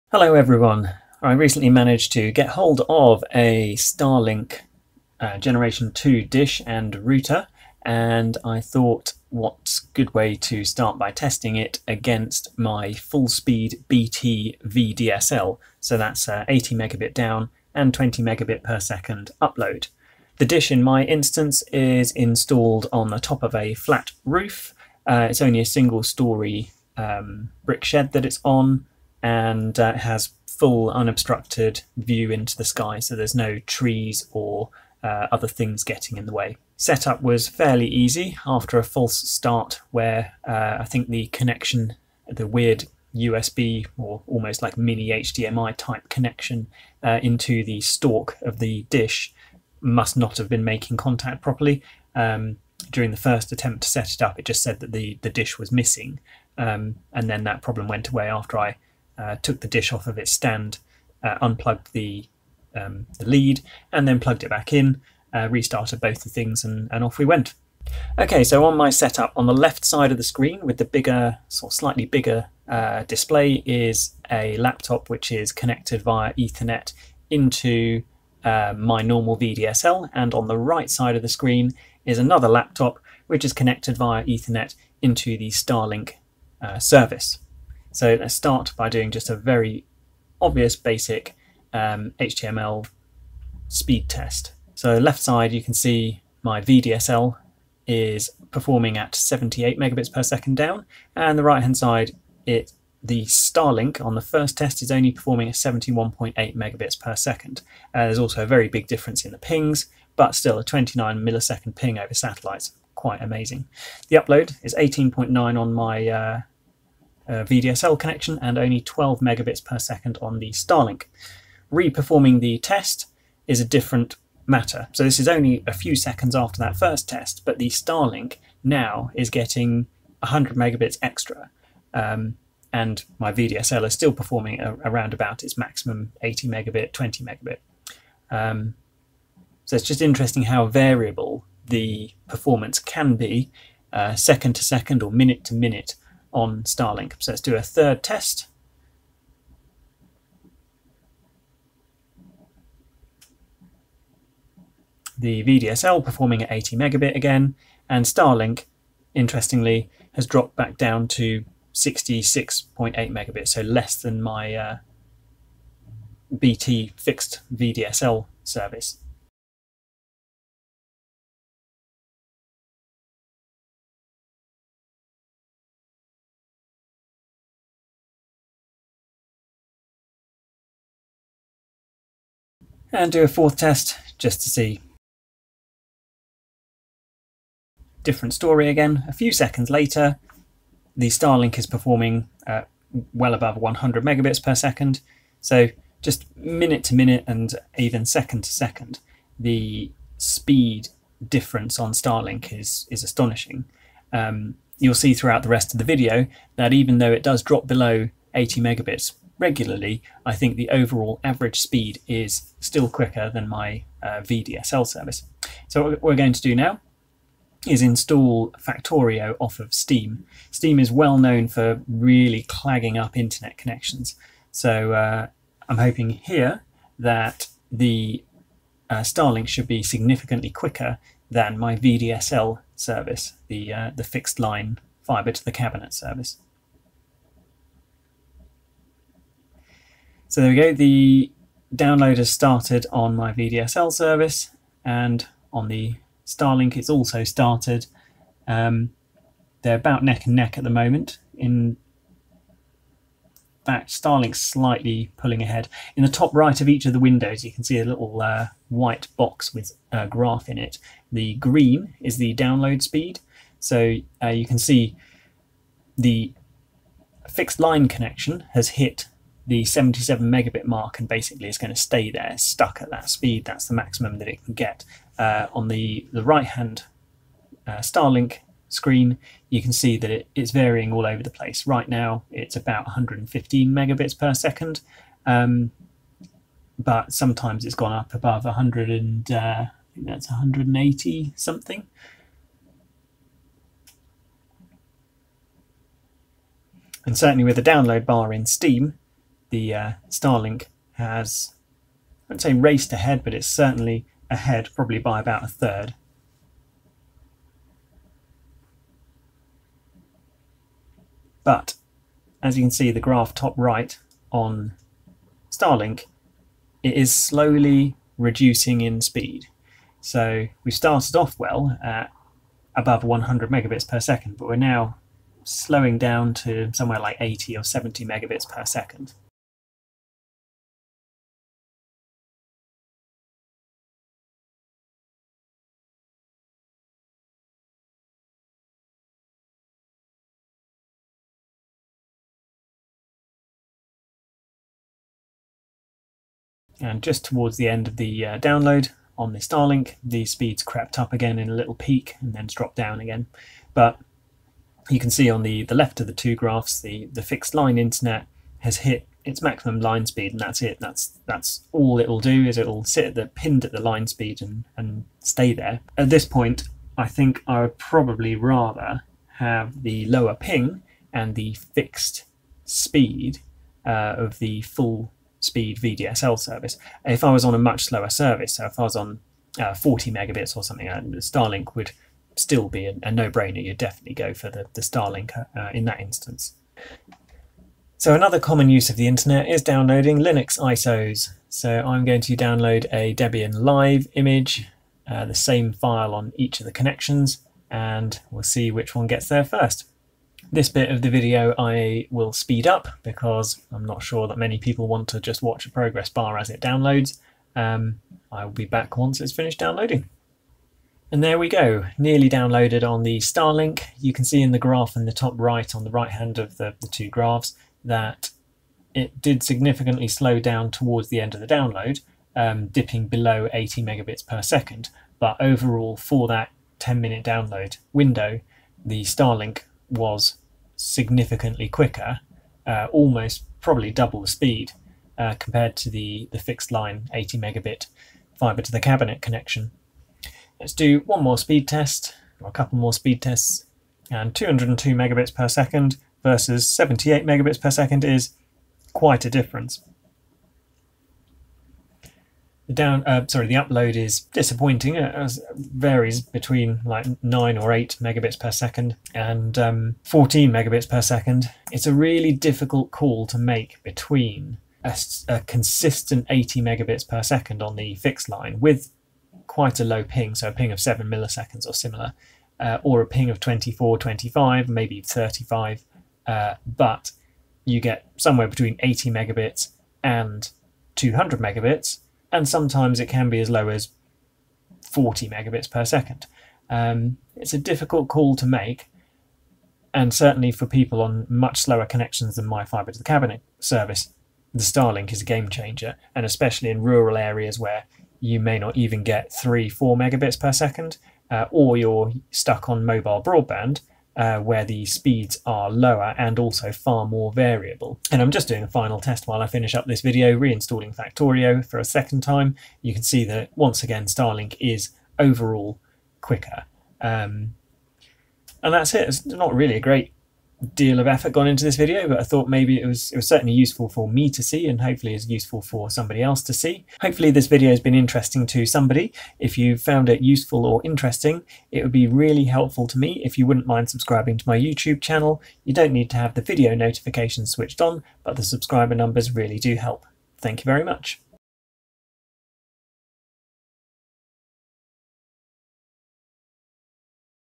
Starlink vs. UK ISP BT FTTC - Steam Game Downloads; Debian ISO and Speed Tests
tl;dw: Starlink averages faster but is far more variable. Starlink costs a lot more per month and up front.
How does SpaceX Starlink stand up to full speed UK Internet Provider BT FTTC (Fibre to the Cabinet).
This video shows the full download experience comparing 80mbps down, 20mbps up FTTC against Starlink with full view of the sky at around 10pm on a Friday evening.
The tests include downloading a Debian live image, Factorio from Steam and lots of web browser based speed tests.